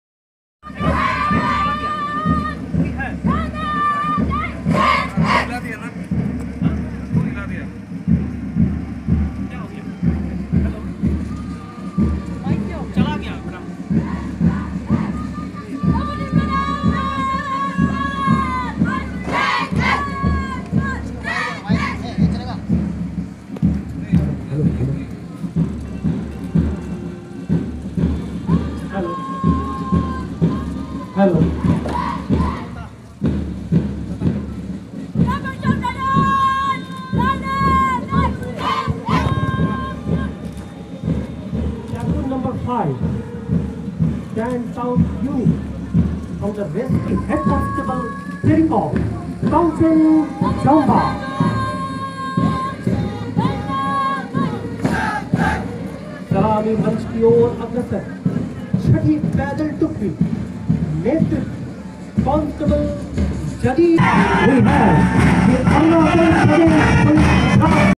practice women cadets
Ti is most powerful women cadets police.so smart and hardly practice.womenmilitarypolice . Womena arunachalpradesh.